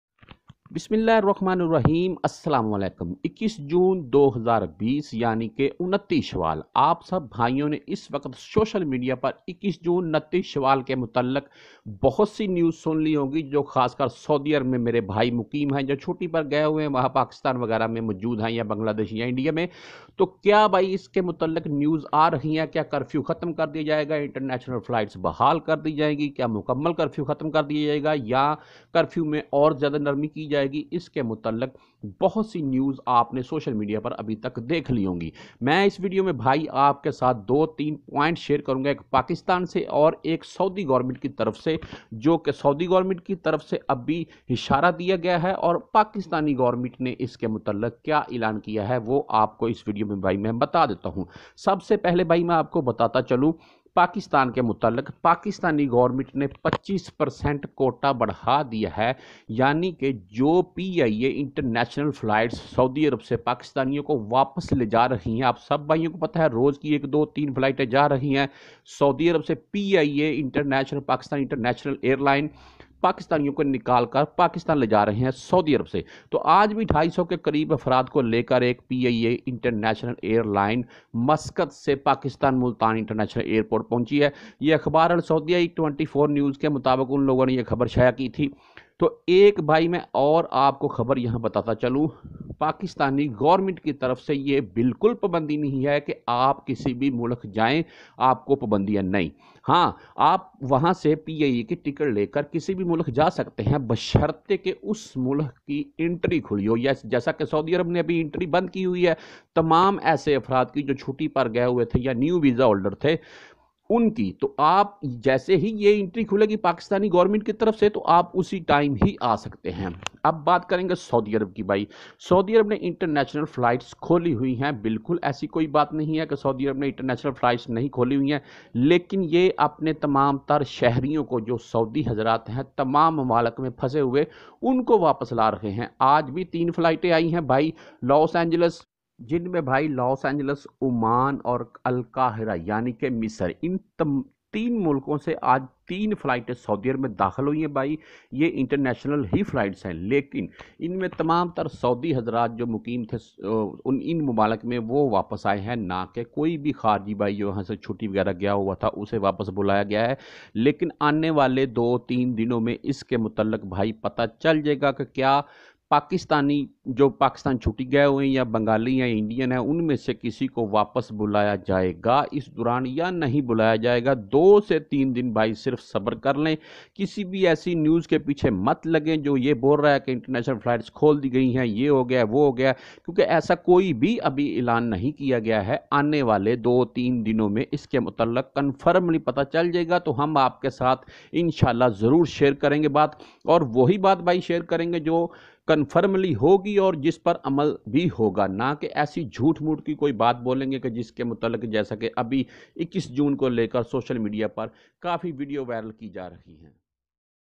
Will international flights be opened in saudi arabia from june 21?, saudi arabia news today, da news
Will international flights be opened in saudi arabia from june 21?, saudi arabia news today, da news, saudi arab se chothi par gai bhai kia 21 june k baad wapas a sakty hai k nahi , kia saudi arab international flights khool raha hai k nahi,